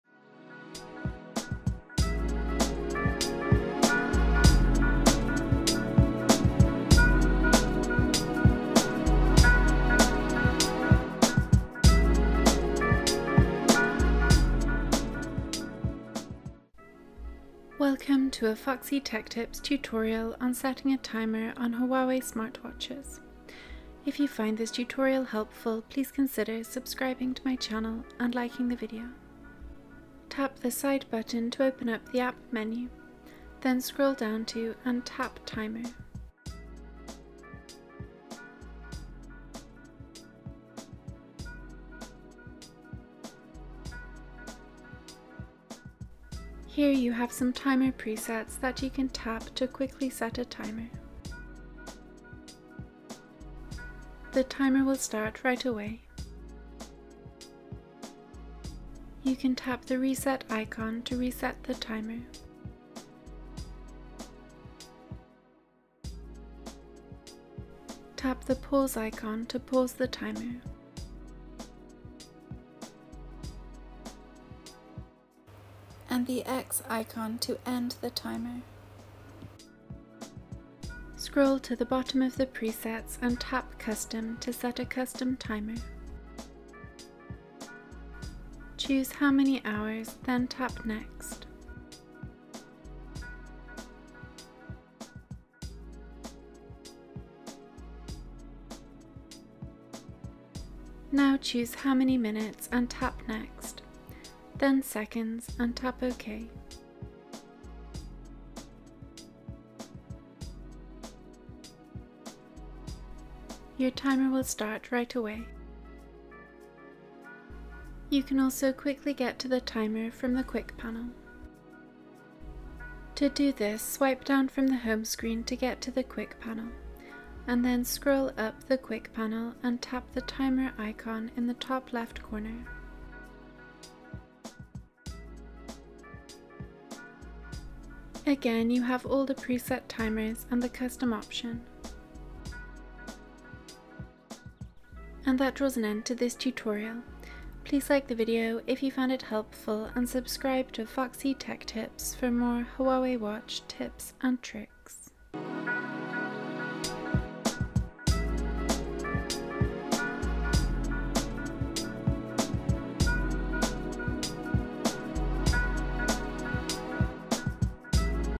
How To Set A Timer On Huawei Band 8
Easy to follow tutorial on setting and managing timers on Huawei Band 8. Want to use the timer on your Huawei smartwatch? Learn how to do it now!

Steps:

Tap the side button to open up the app menu, then scroll down to and tap timer.

Here you have some timer presets that you can tap to quickly set a timer.

The timer will start right away.

Tap the pause icon to pause the timer.

You can tap the reset icon to reset the timer.

Scroll to the bottom of the presets and tap ‘custom’ to set a custom timer.

Choose how many hours, then tap ‘next’.

Now choose how many minutes and tap ‘next’ then seconds and tap ‘ok’.

Your timer will start right away.

You can also quickly get to the timer from the quick panel.

To do this, swipe down from the home screen to get to the quick panel and then scroll up the quick panel and tap the timer icon in the top left corner.

Again you have all the preset timers and the custom option.